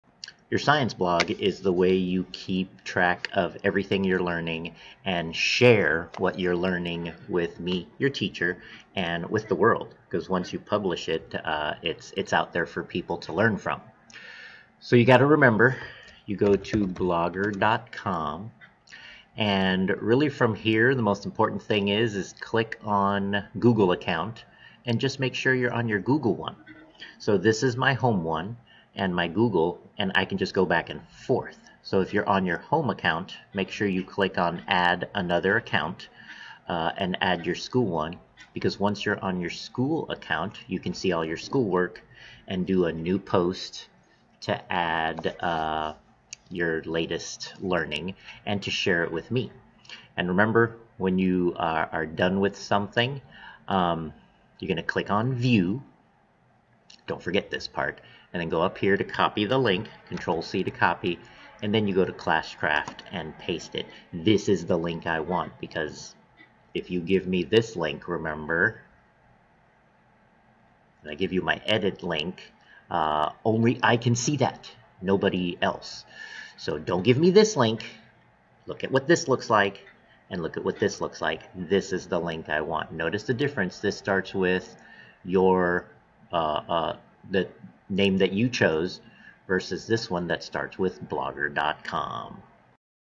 Accessing School Blogger Account
Showing how students can access their school blogger accounts from home to share their learning and submit assignments.